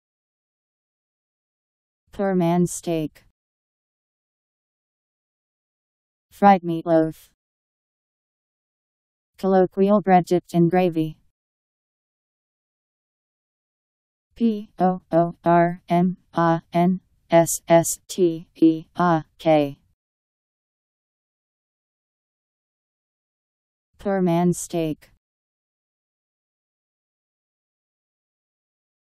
Poor man's steak Meaning
Video shows what poor man's steak means. fried meatloaf.. (colloquial) bread dipped in gravy..  Poor man's steak Meaning. How to pronounce, definition audio dictionary. How to say poor man's steak. Powered by MaryTTS, Wiktionary